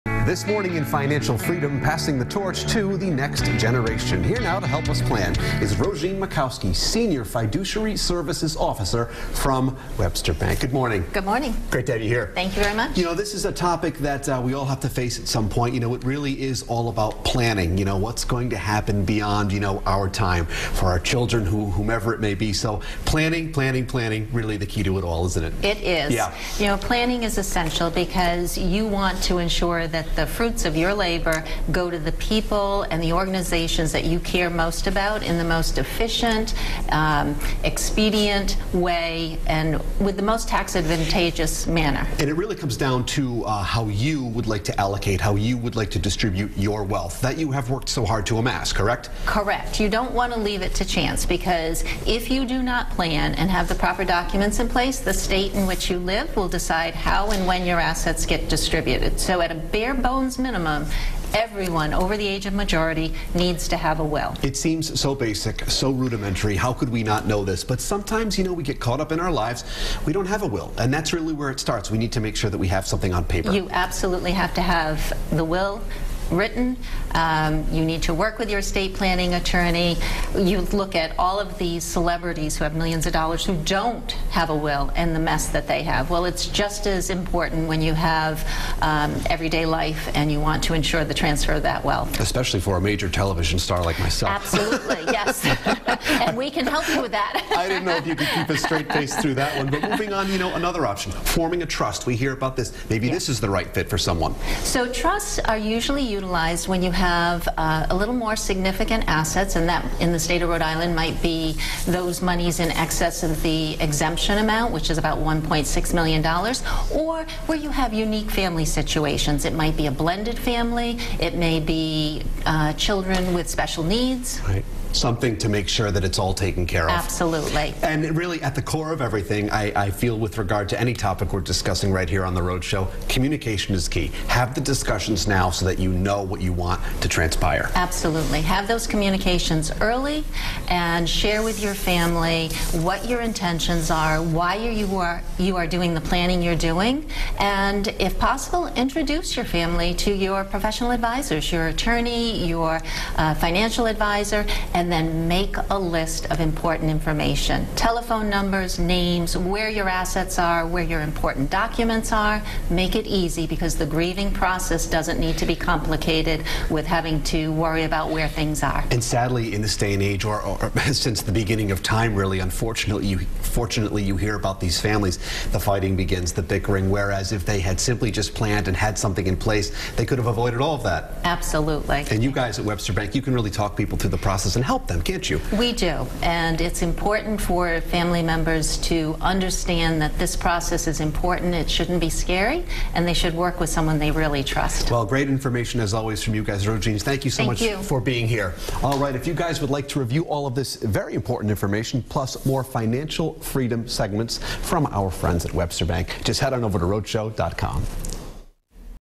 Passing the Torch to the Next Generation
Webster Bank joins us with more on passing your finances on to the next generation.